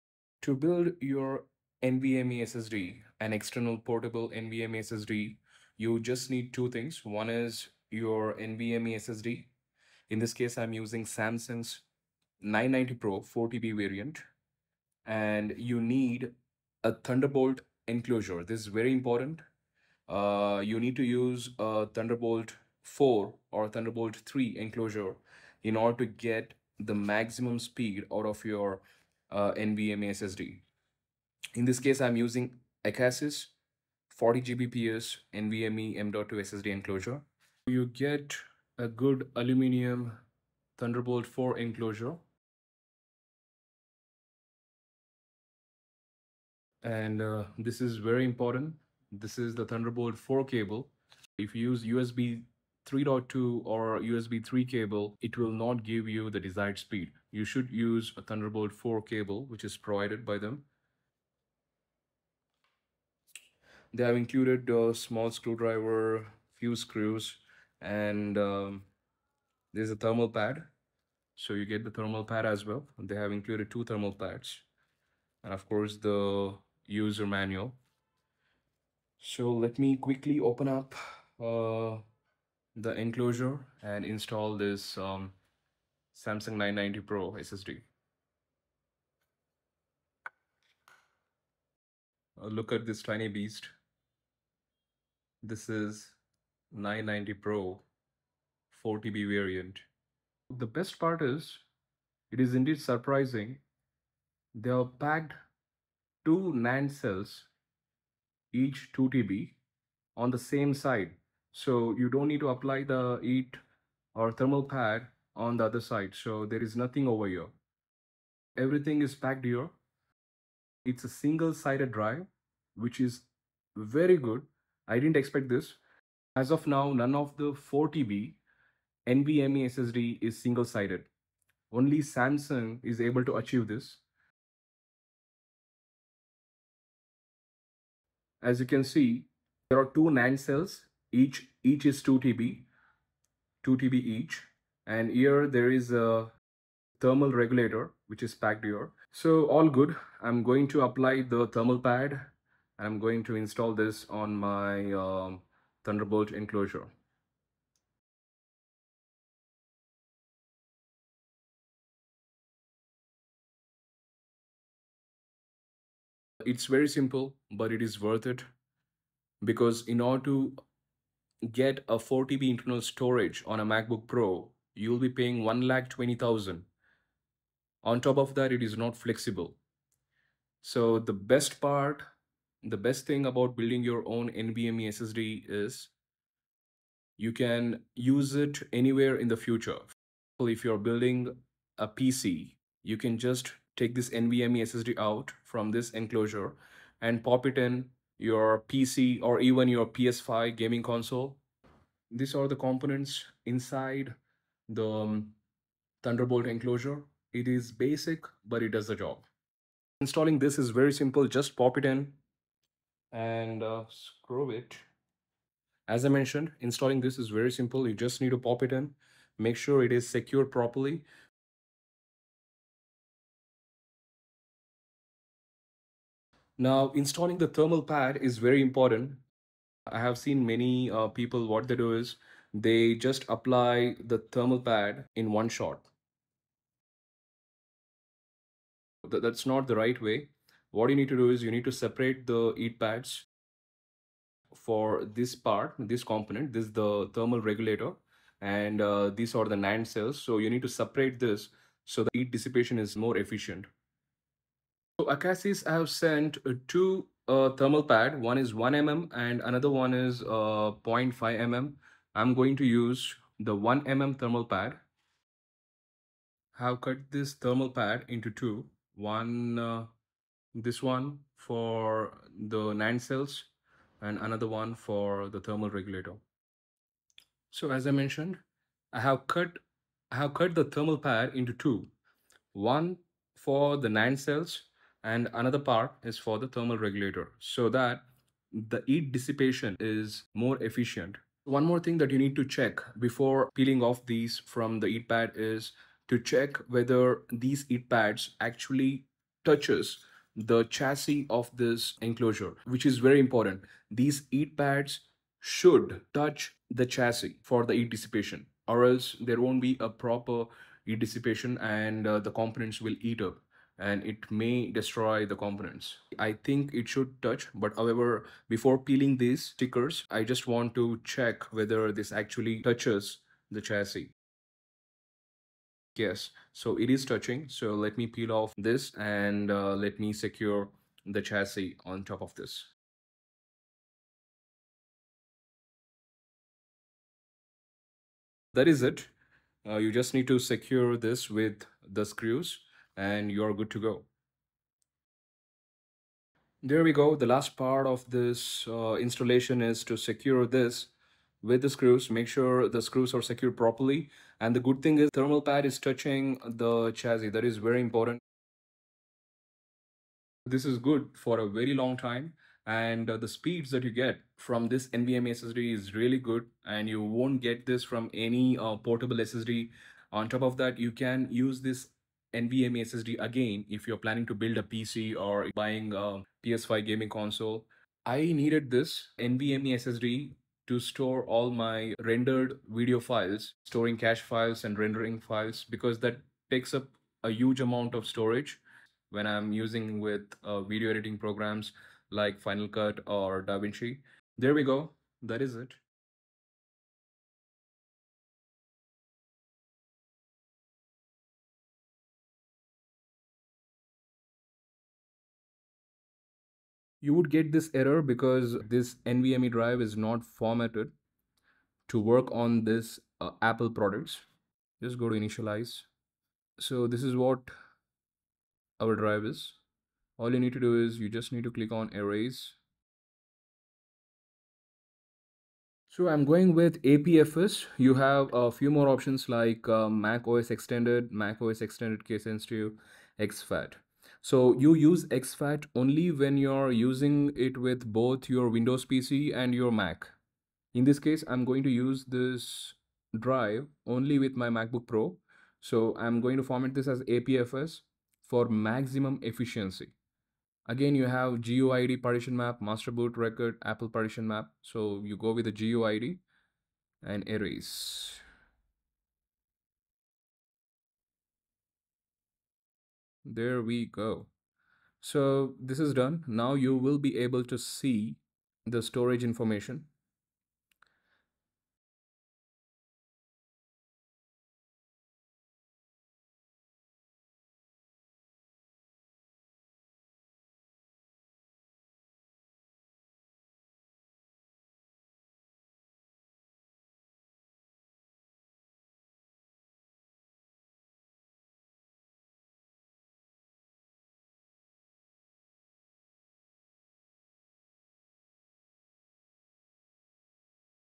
SAMSUNG 990 Pro 4TB NVME SSD WITH ACASIS TBU405 THUNDERBOLT ENCLOSURE
The powerhouse combination of the Samsung 990 Pro 4TB SSD paired with the Acasis Thunderbolt enclosure, whether you're a content creator, professional gamer, or simply crave blazing-fast storage, this duo is about to revolutionize your workflow!

In this video, I will demonstrate the setup process, run performance tests, and showcase real-world use cases to illustrate just how much of a difference this combination can make in your daily computing tasks.

From 4K video editing to large-scale data transfers, the Samsung 990 Pro SSD with the Acasis Thunderbolt enclosure is ready to handle anything you throw its way.

I am using this with my M1 Pro Macbook Pro.

SAMSUNG 990 PRO 4TB NVME SSD
ACASIS 40 Gbps THUNDERBOLD ENCLOSURE
MACBOOK PRO M1 PRO